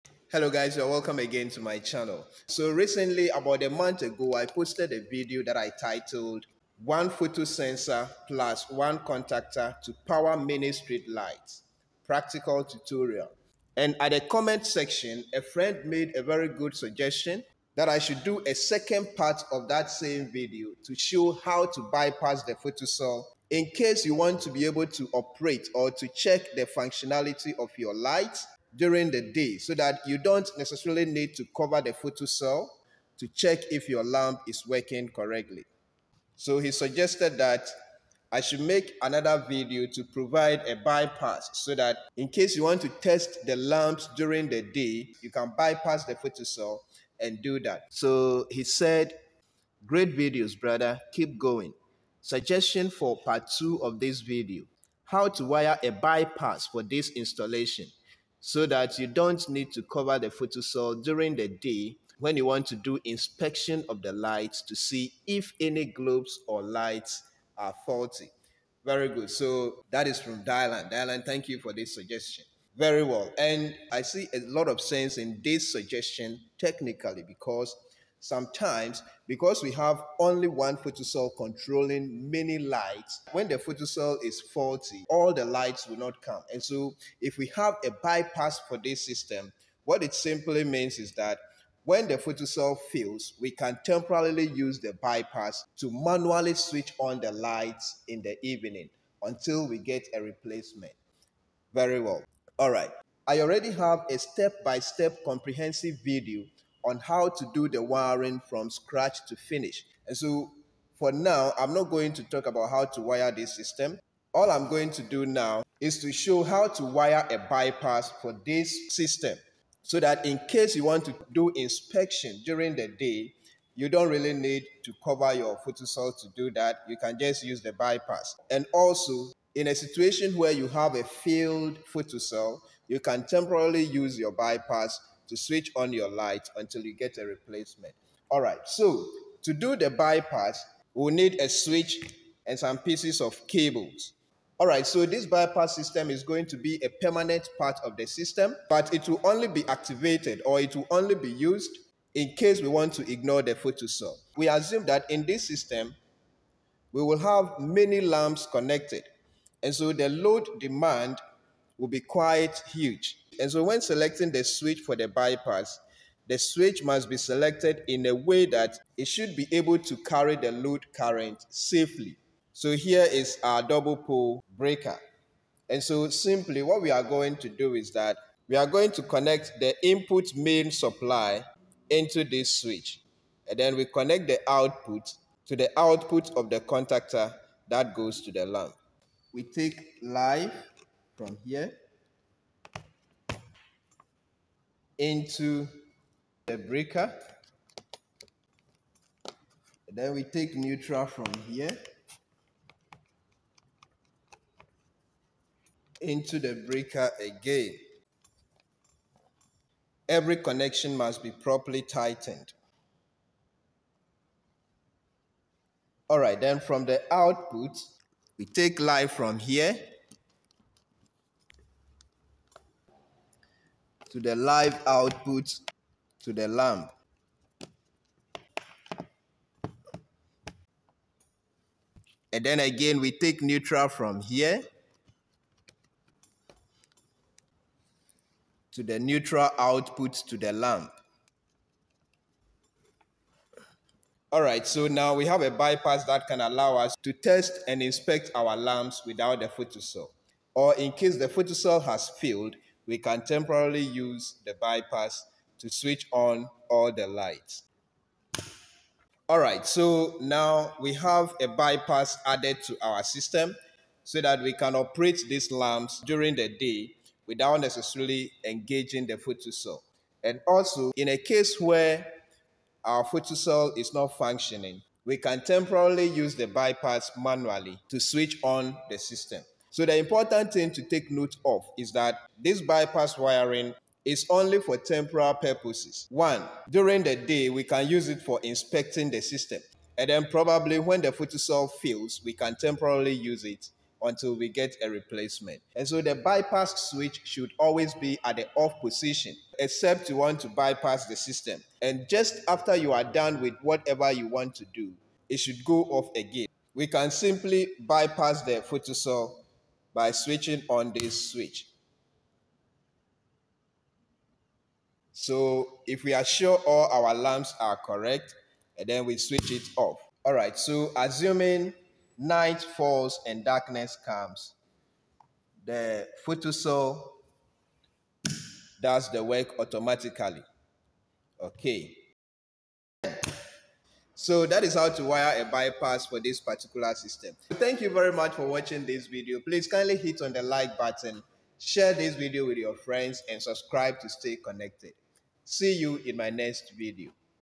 Photocell bypass wiring
learn how to properly wire a bypass for a photocell. Whether you're installing outdoor lighting or troubleshooting an existing setup, understanding photocell bypass wiring is crucial. Watch and learn the step-by-step process of making the necessary connections.